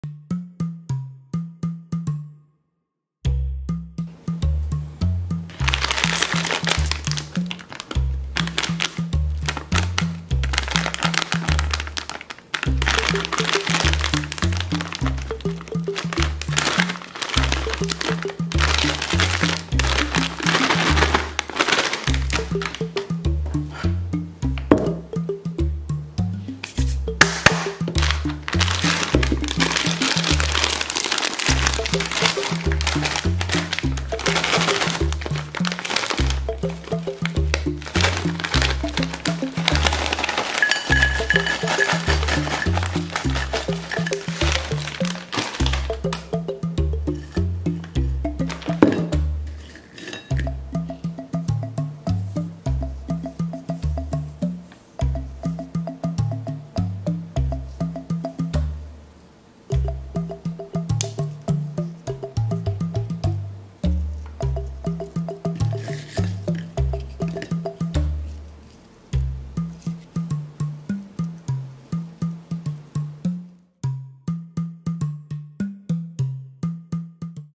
Takis Blue Heat Chips
This is the unwrapping of the Takis Blue Heat potato chips. It will show the contents inside along with a visual and sound perspective.

Takis Blue Heat

Code: A7WHUMJNTOVZSGXP